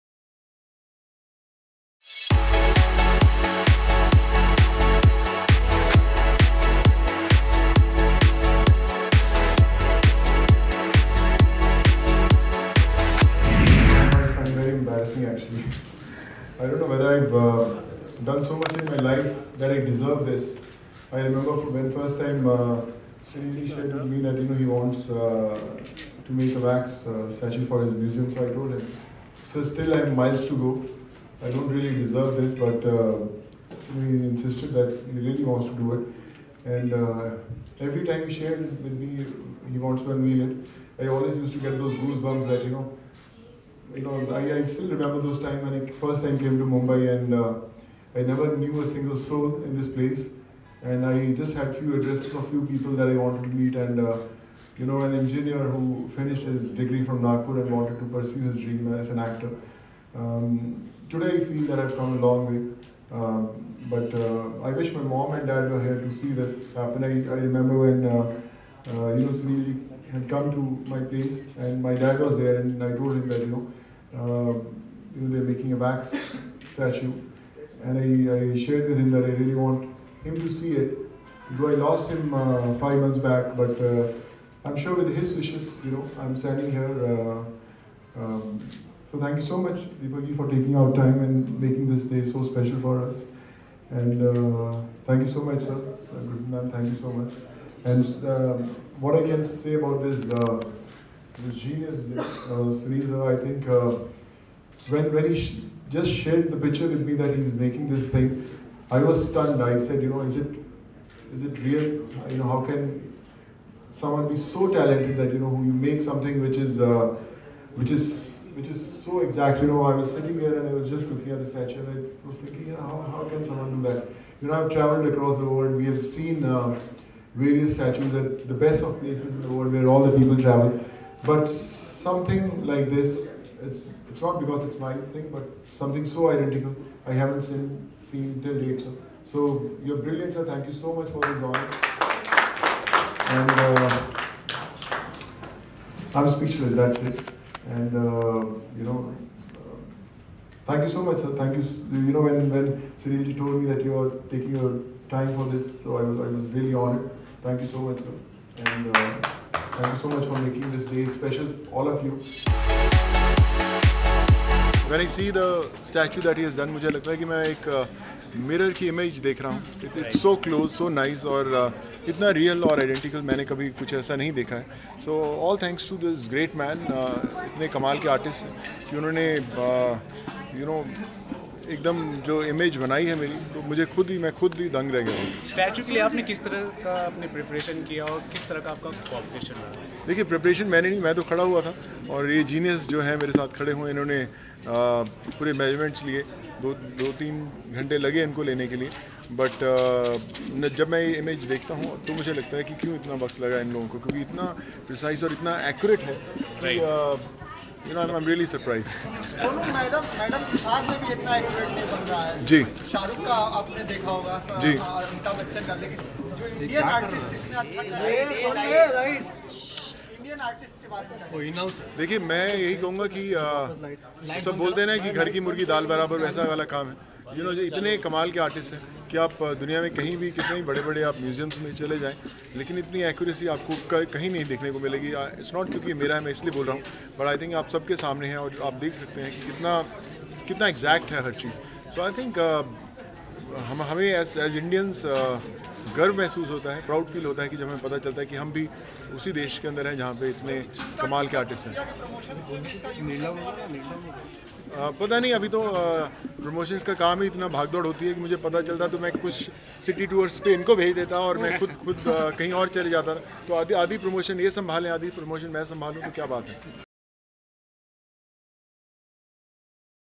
UNBELIEVABLE ! Sonu Sood Replica Meets Actor Sonu Sood
What happened when Bollywood actor Sonu Sood met his duplicate at an event in Mumbai. Watch the video.